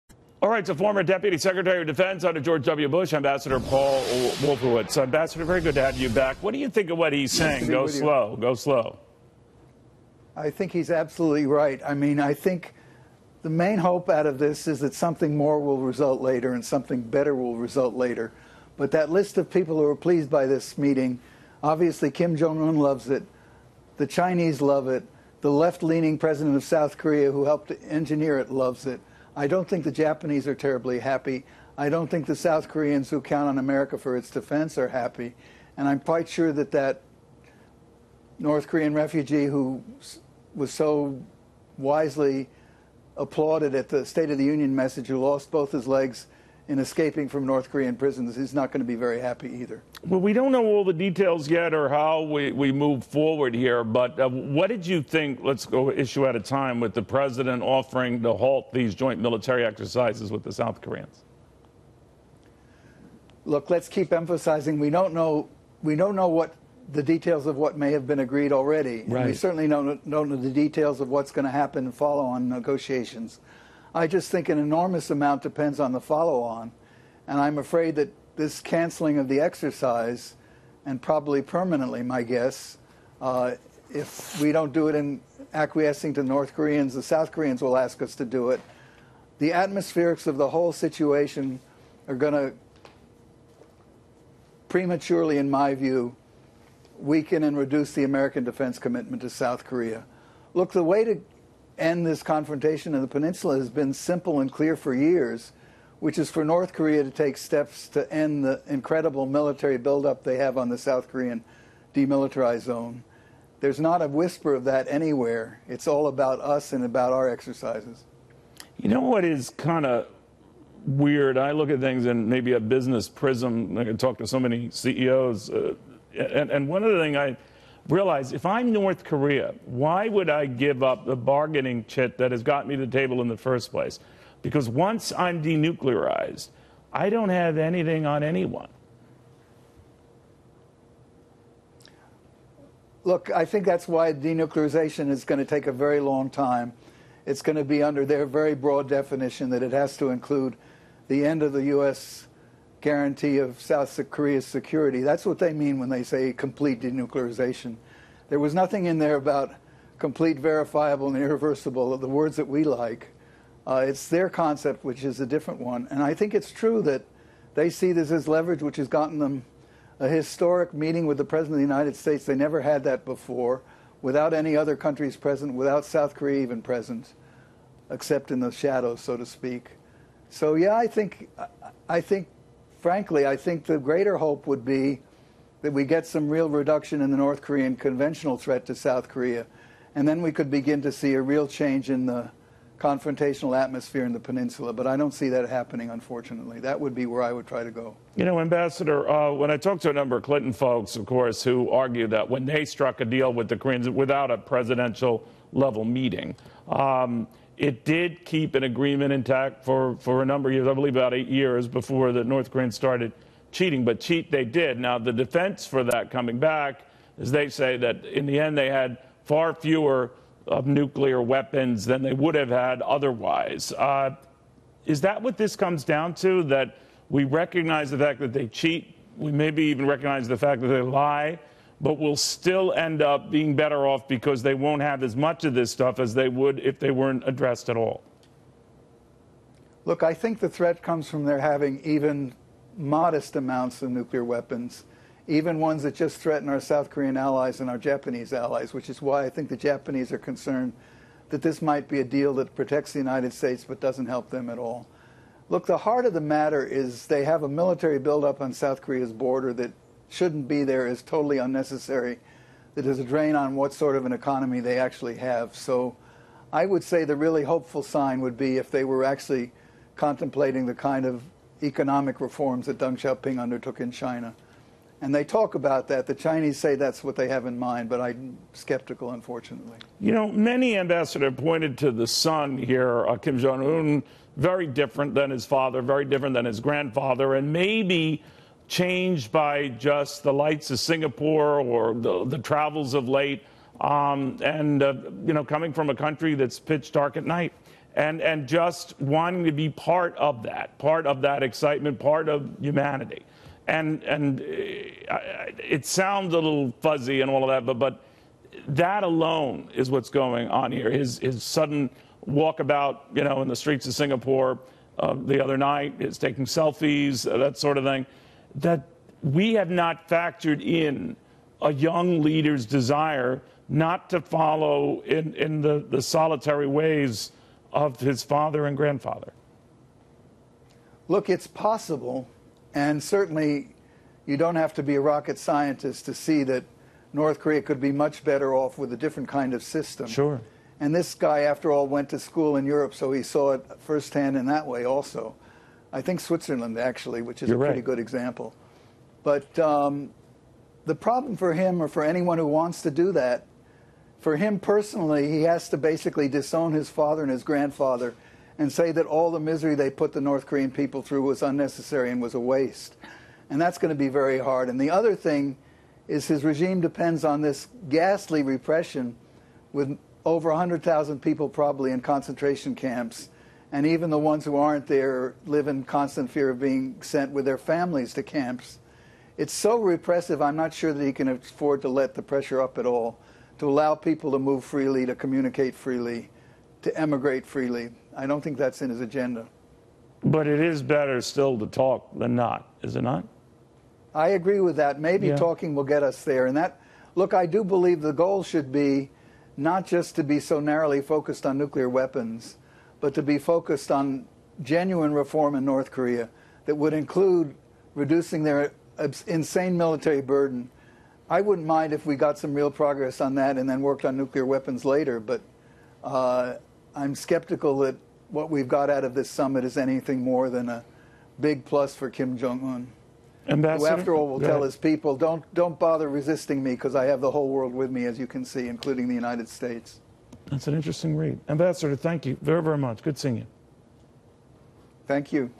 Denuclearization will take a very long time: Ambassador Wolfowitz
Former U.S. Deputy Secretary of Defense Ambassador Paul Wolfowitz on North Korea’s vow to give up its entire nuclear program.